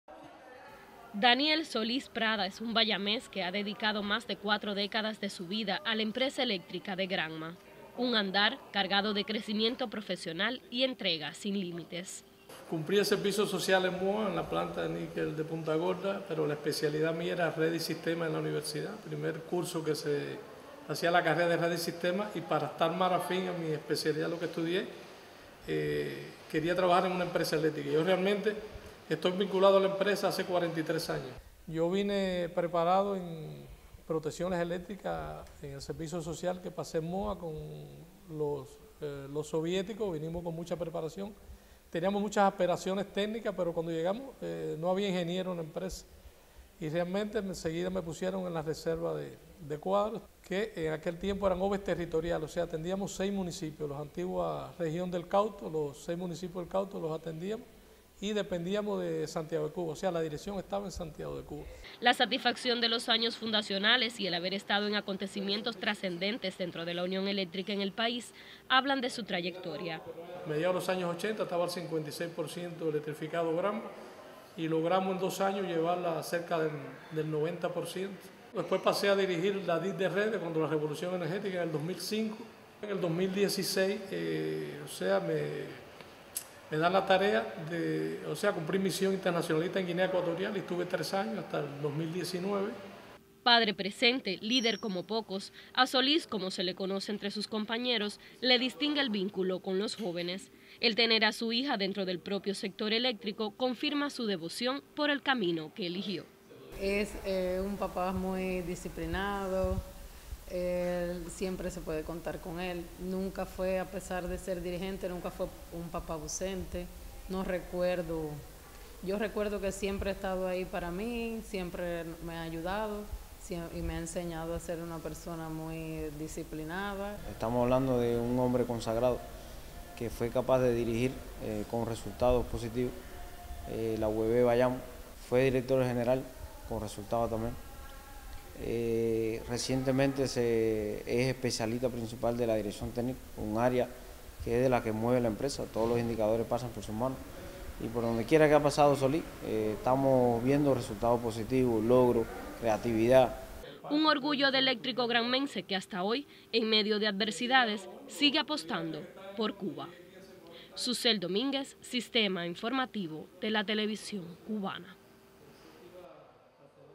Sobresale trabajador eléctrico de Granma por más de 40 años de labor
Daniel Solís Prada es un trabajador de la Empresa Eléctrica de la provincia
 Granma que atesora más de 40 años de labor ininterrumpida dedicados a este sector.   Sobresalen en su trayectoria el haber aportado a  acontecimientos trascendentales para la Isla en materia
energética y enfrentarse desde muy joven al liderazgo.